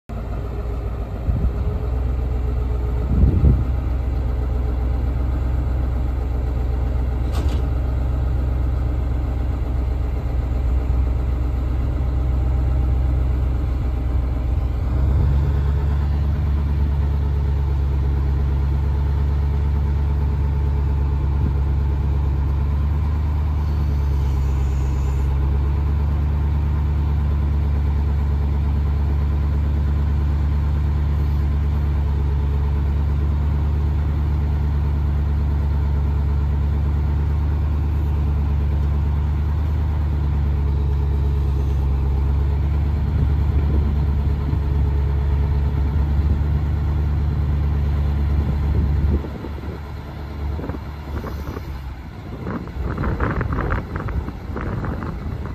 1989 VERSALIFT SHV36PI For Sale
Year: 1989 Make: GMC Model: C7000 Vehicle Type: Truck Mileage:43,742; Hours:2,880; Bucket Hours:656 Body Type: Medium Conventional Cab & Chassis Drive Line: 4x2 Engine Type: V8, 8.2L Fuel Type: Diesel Transmission: 5-Speed Manual VIN #: 1GDK7D1F3KV504445  Features and Notes: GM 8.2L DIESEL ENGINE, 5-SPEED MANUAL TRANSMISSION, AIR BRAKES, PTO, MANUAL DOORS, MANUAL WINDOWS,SINGLE AXLE DUAL WHEELS, PINTLE HITCH, BAKER EQUIPMENT SERVICE BED, SN-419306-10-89, MODEL# 1151-28,VERSALIFT BUCKET, MODEL- SHV-36-P1, SN-138926, 36' PLATFORM HIEGHT, 300LBS CAP,BUCKET CONTROLS,TELEPHONE COOP UNIT **LIMITED FUNCTION CHECK~CRANKS AND RUNS**  #121454